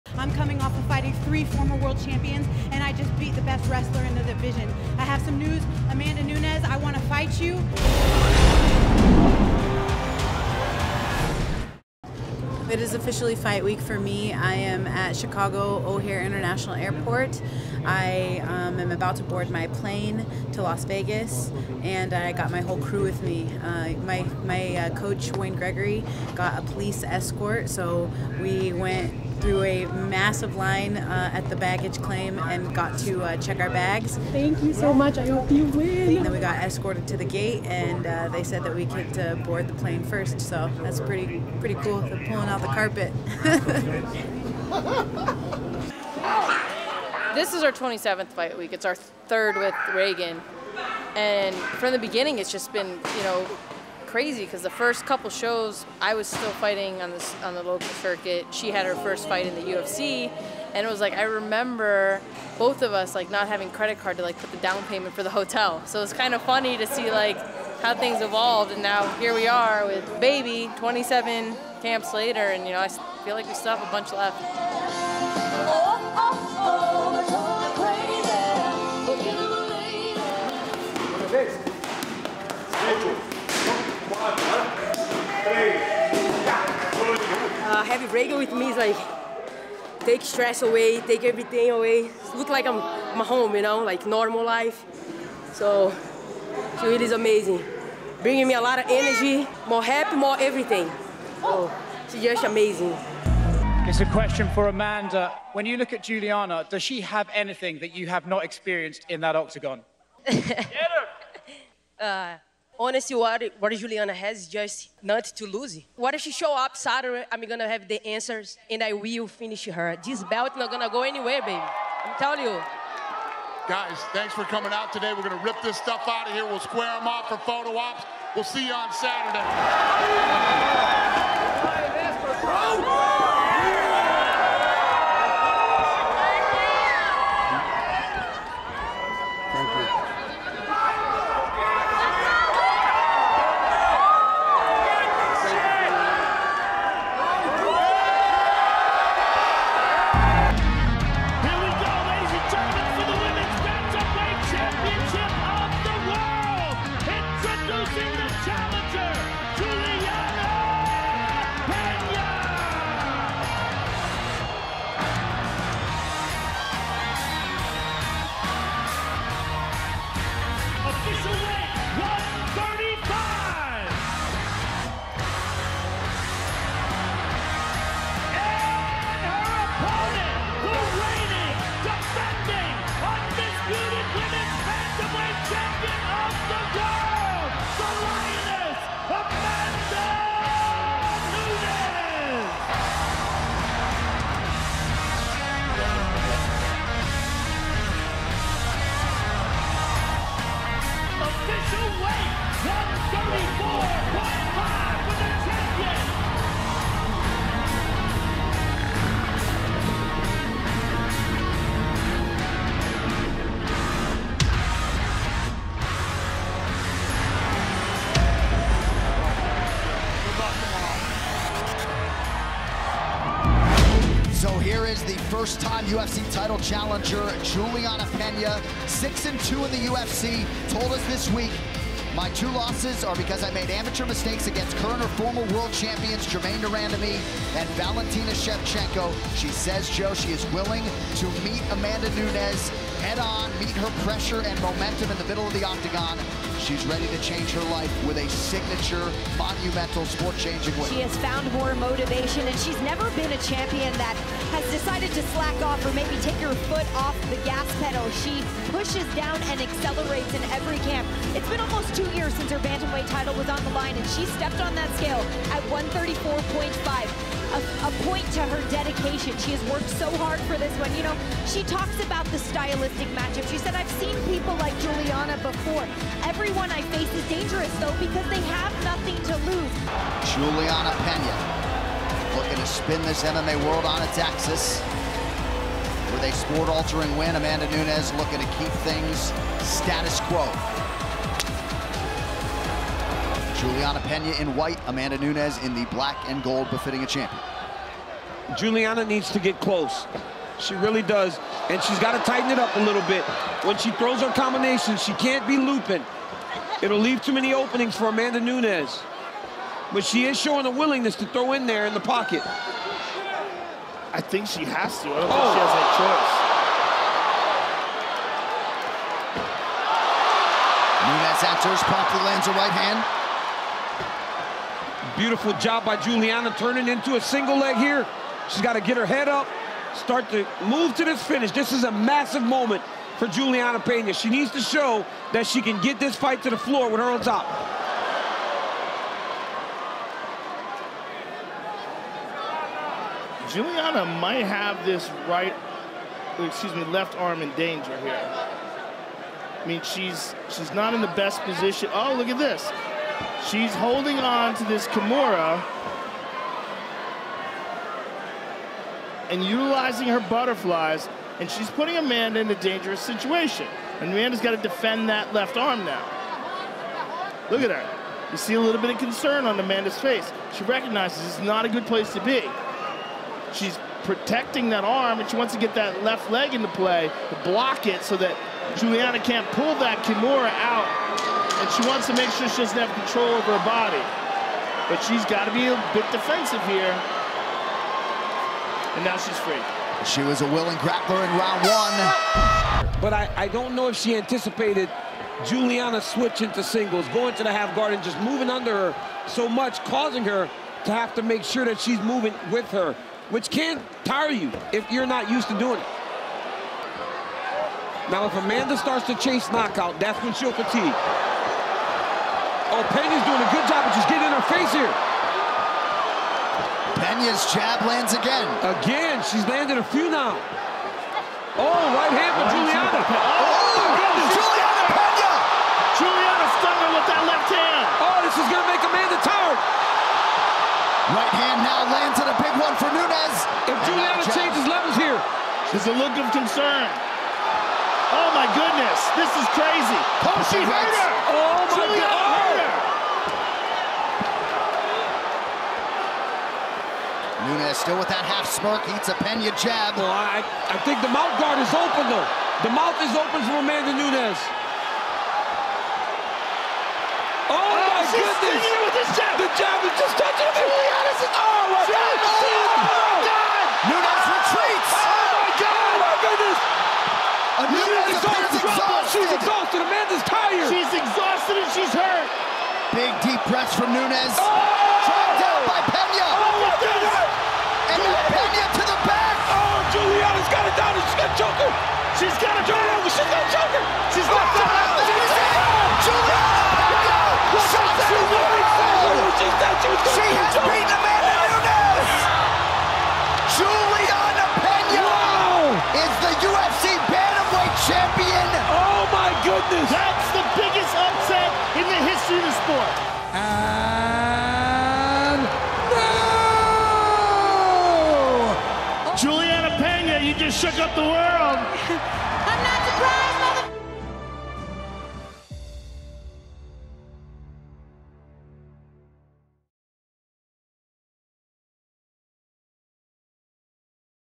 In Depth | Julianna Peña vs Amanda Nunes 1
Go in depth on act one between current bantamweight queen Julianna Peña and featherweight champion Amanda Nunes that took place back at UFC 269. Peña shocked the world when she earned the second-round submission victory over Nunes to claim the title, setting the scene for the two competitors to run it back at UFC 277 on Saturday.